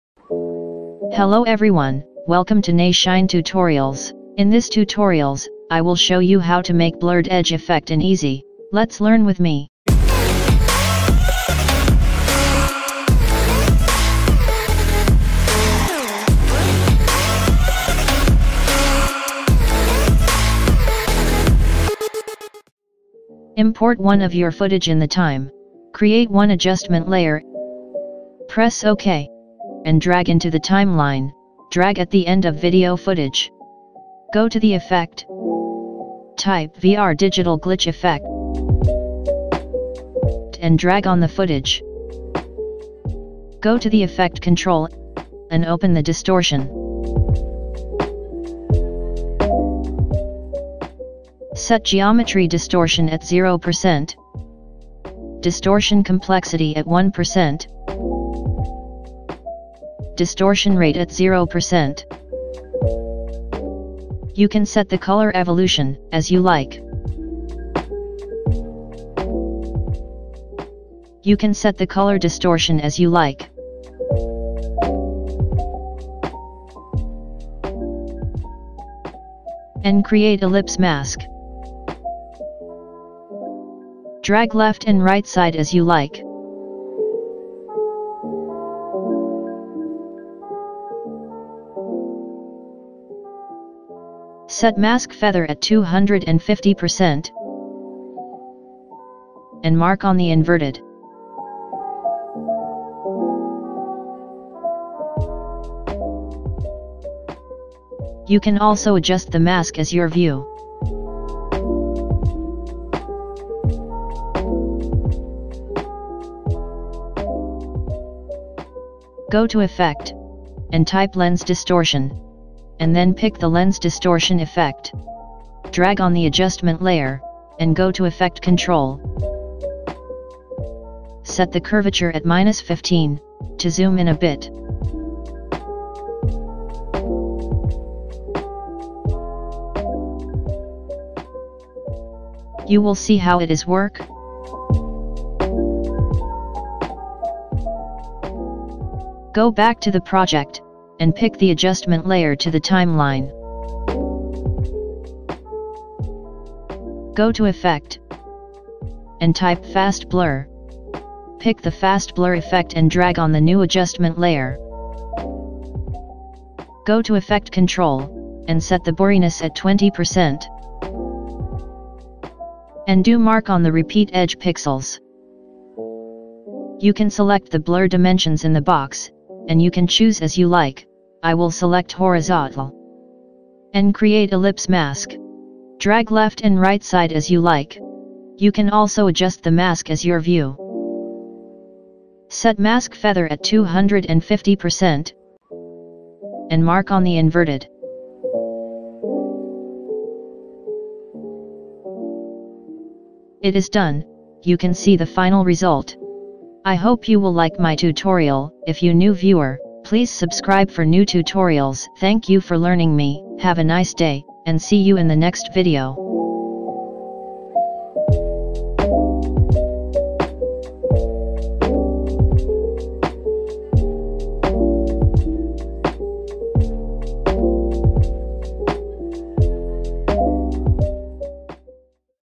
Trippy Blurred Edge Effect | Dreamy Look in Video - Adobe Premiere Pro Tutorials
In this video, I show you how to create blurred edges and RGB distortions using Premiere Pro CC. I hope that this video helps take your videos to the next level!

►Watch this
Amazing Video Color Grading in 1 Click | Fastest Way Of Color Grading | Adobe Premiere Pro Tutorials

You can download footage from Pexels for free!
Video Footage Credited _ Video from Pexels